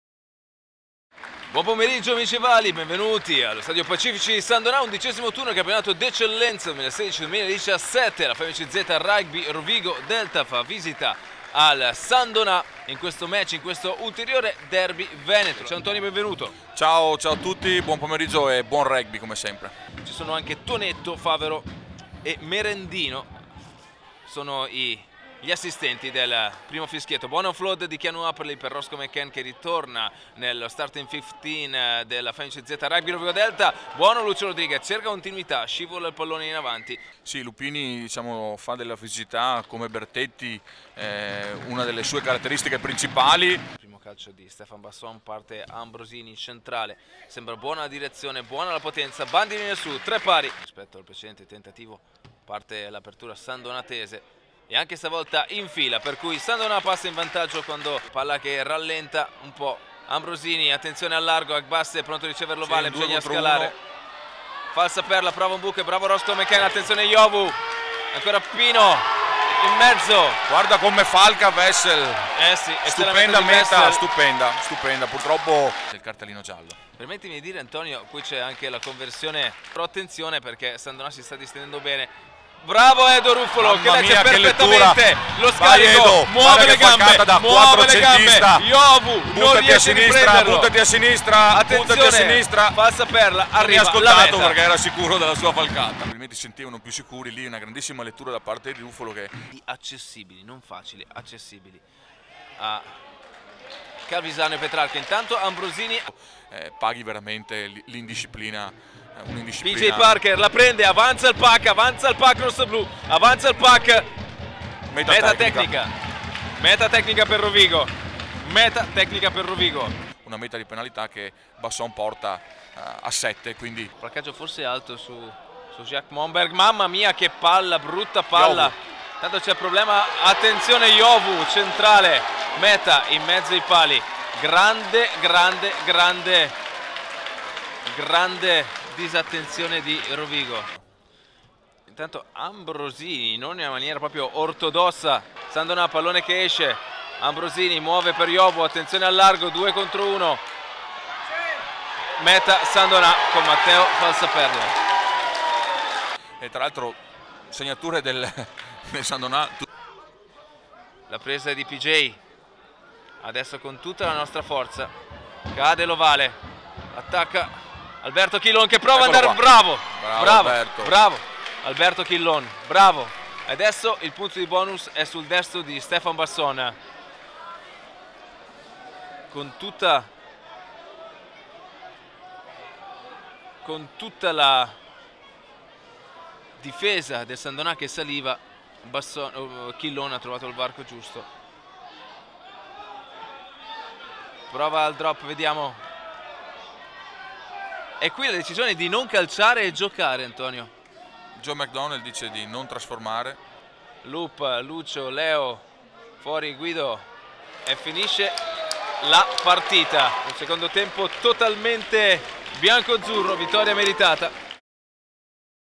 Highlights / Lafert San Donà - Femi-CZ Rovigo Delta / 18-02-2017
2^ giornata di ritorno - Campionato Nazionale d'Eccellenza 2016/2017